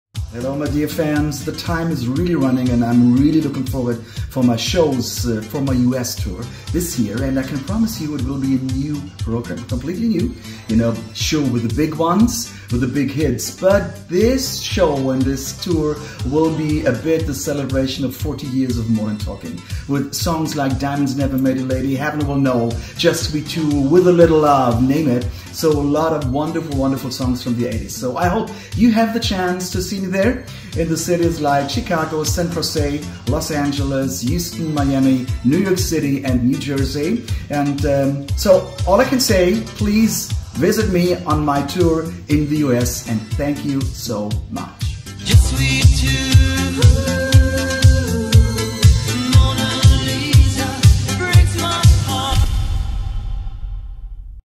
Greetings from Thomas Anders - 2024 USA Tour
🚨New set prepared for the 40th Anniversary of Modern Talking- Check out Thomas greetings about the tour! Tour information Thomas Anders USA 2024 Tour featuring Sabrina Salerno & Precious Wilson Coming to Chicago, San Jose, Burbank (Los Angeles), Houston, Miami, NYC, NJ Visit Thomas Anders USA dot COM for complete details.